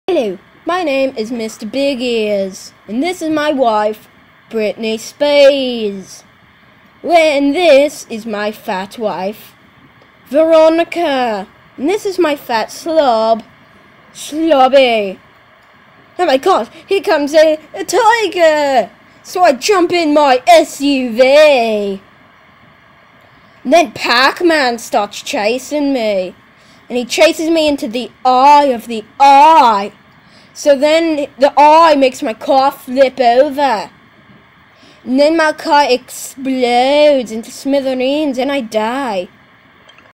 Mr.Big Ears
A dog has 3 wives and gets chased by a tiger and dies